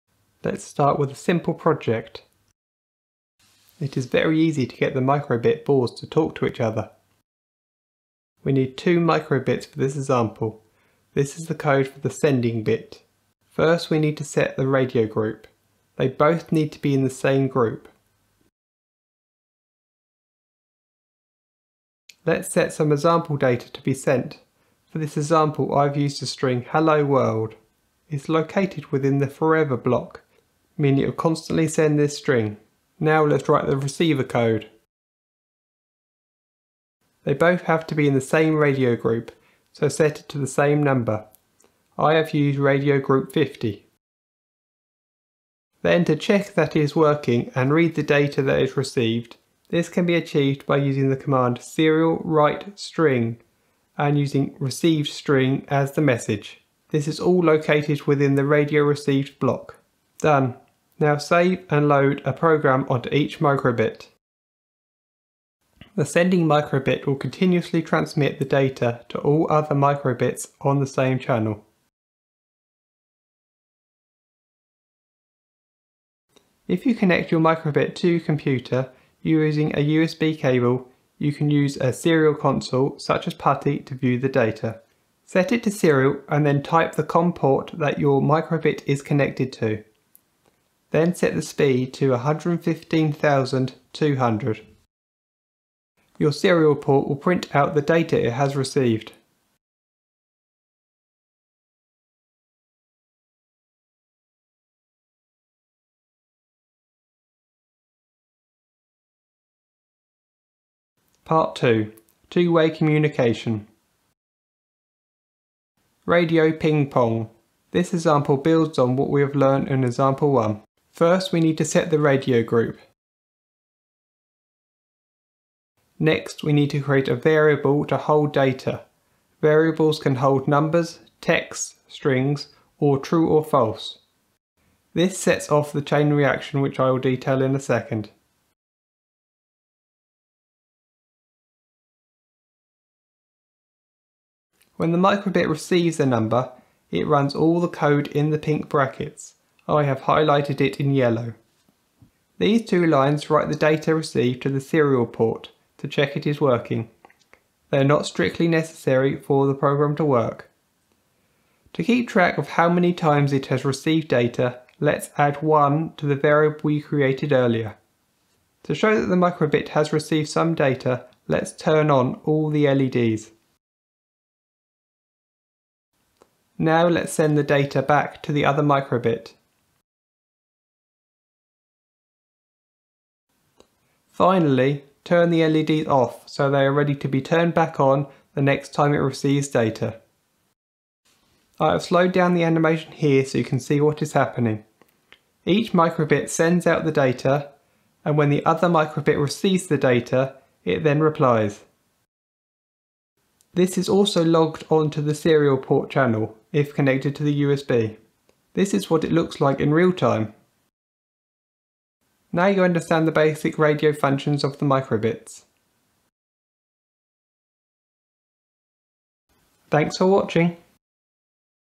How to use the Radio function on BBC Micro:bit
Tutorial on how to use both 1 way and 2 way radio on the BBC Micro:bit. Simple tutorial with animation and explanation of the code for each programme.

With both a voice over and text on screen it is accessible to everyone.